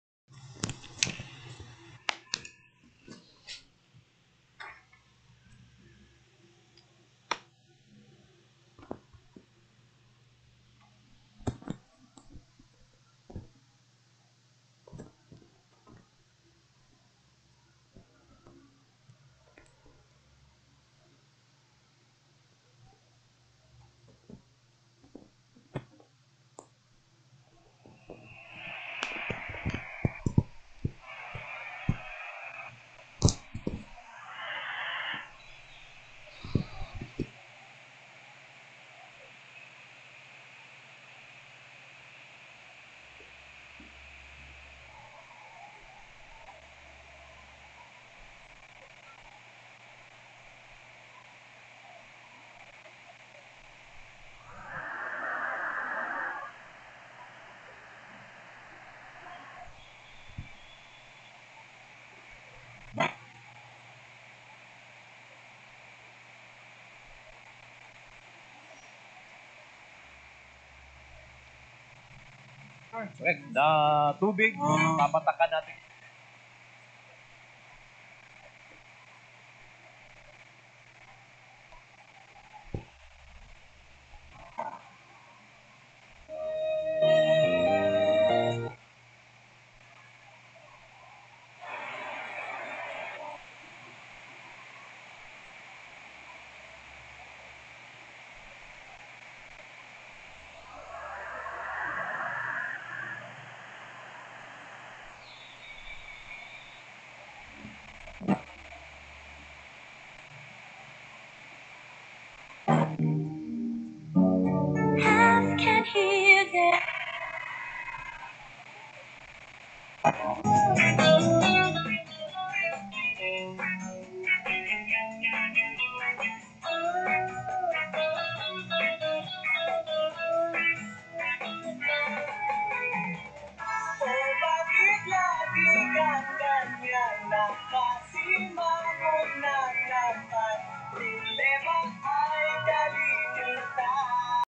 FM Signal (November 11 at 4:19 AM)
Result: 88.3 xfm, 89.9 memories, 90.7 brigada, 93.9 ifm, 94.7 energy fm, 99.1 ifm, 99.5 barangay rt, 103.1 radyo bandera (off air), 104.7 dream radio
Receiver: Sihuadon R-108 (mod)
Note: Still get weak their Cebu station, I need to fix this error